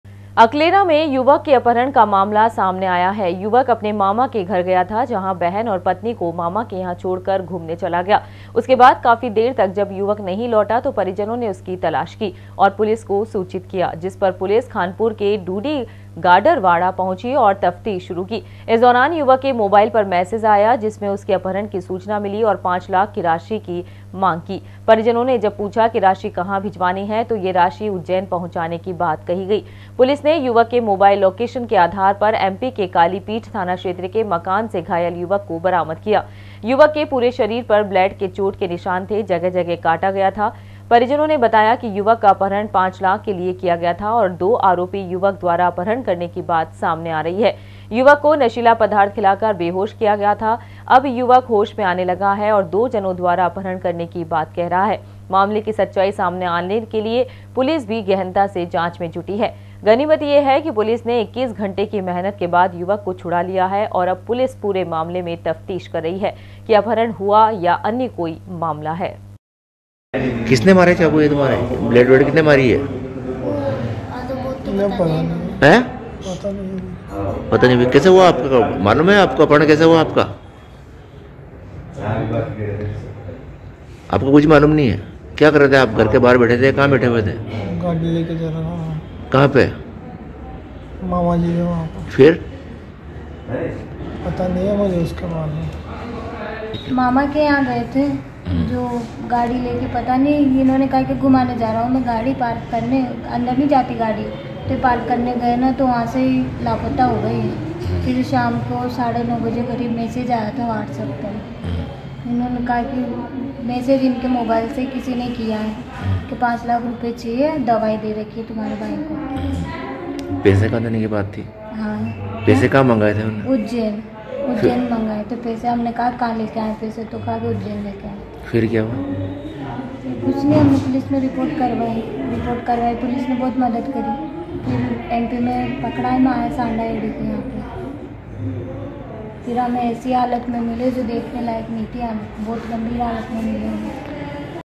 21 november 2020 @ news # झालावाड़ - फिरौती के लिए युवक का अपहरण
21 november 2020 @ news # झालावाड़ - फिरौती के लिए युवक का अपहरण